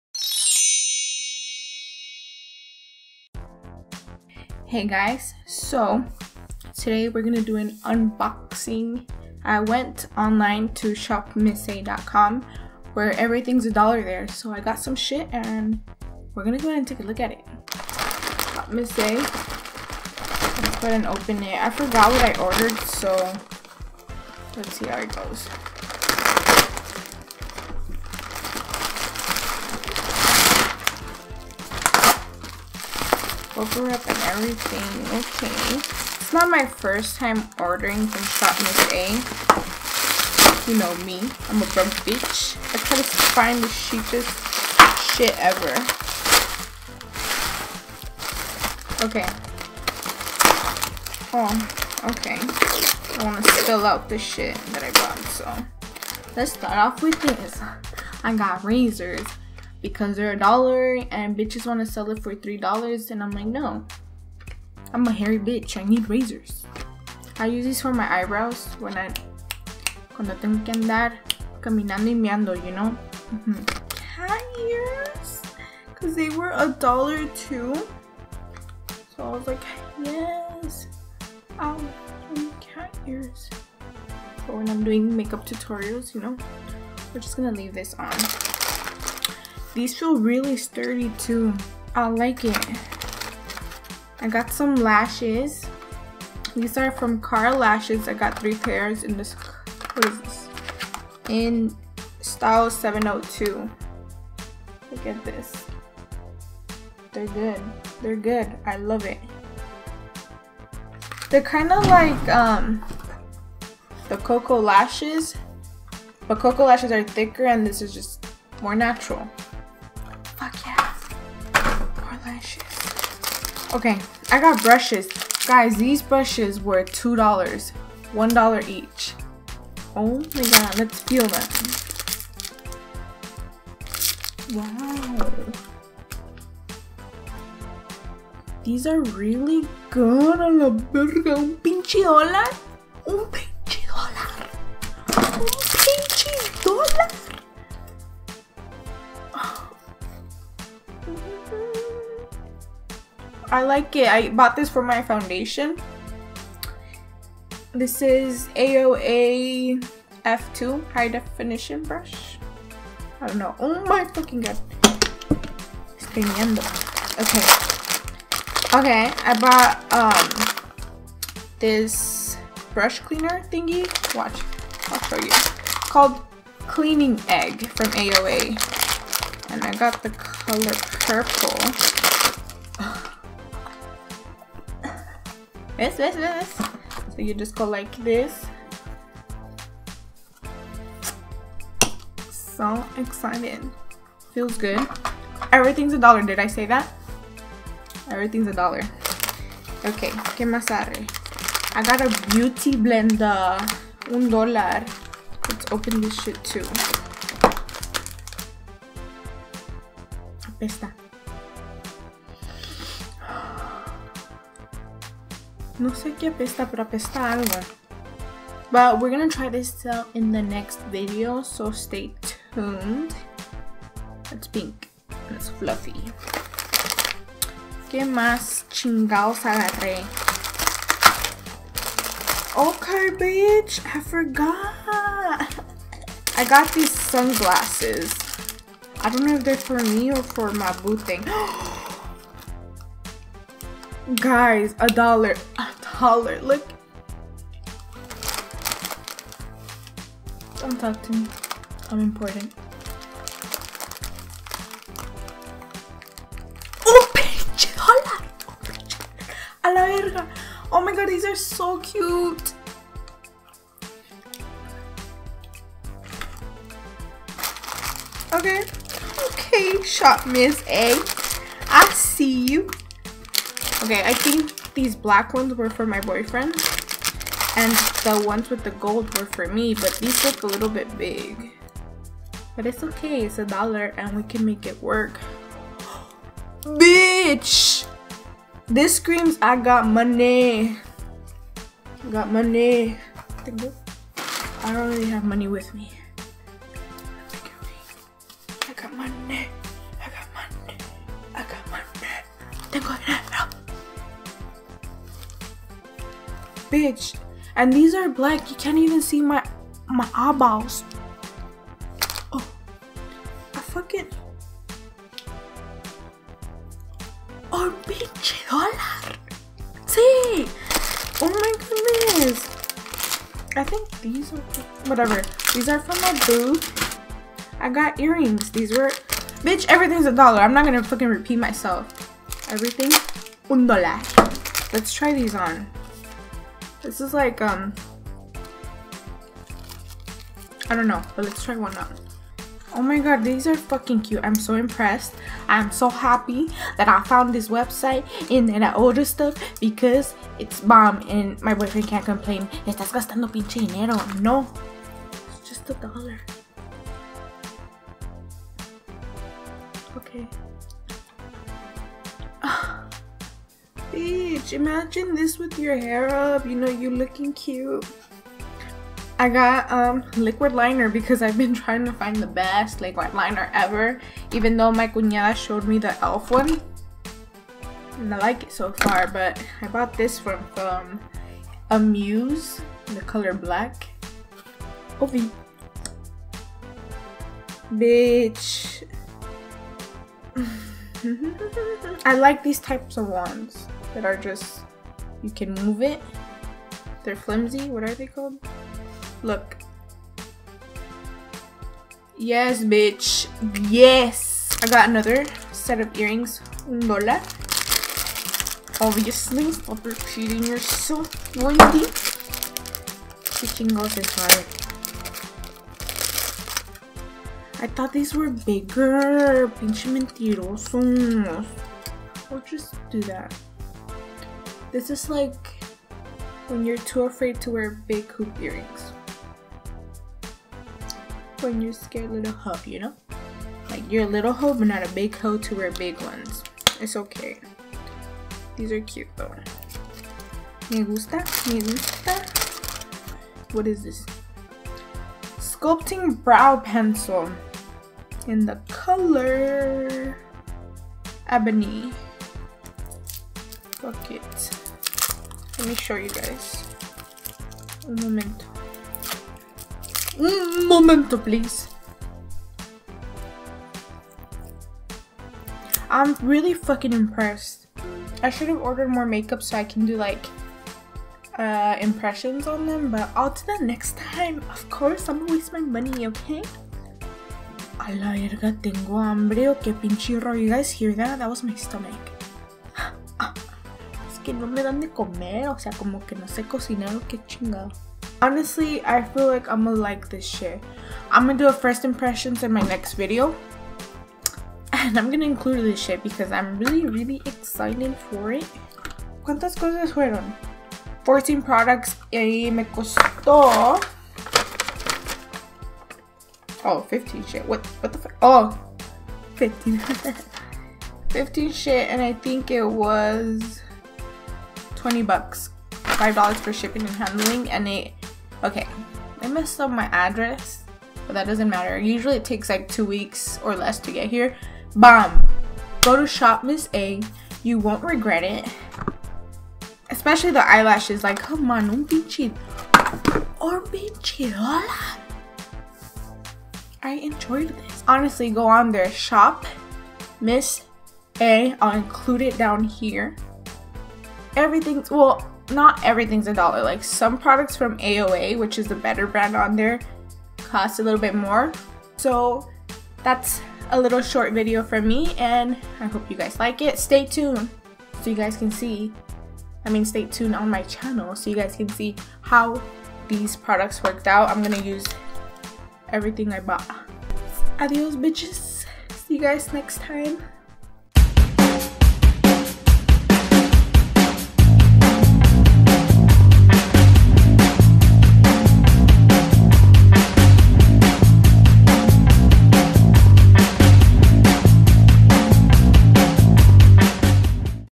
SHOPMISSA UNBOXING
products are from Shopmissa.com go check it out!

also disclaimer i was eating hot cheetos so ignore my fingers.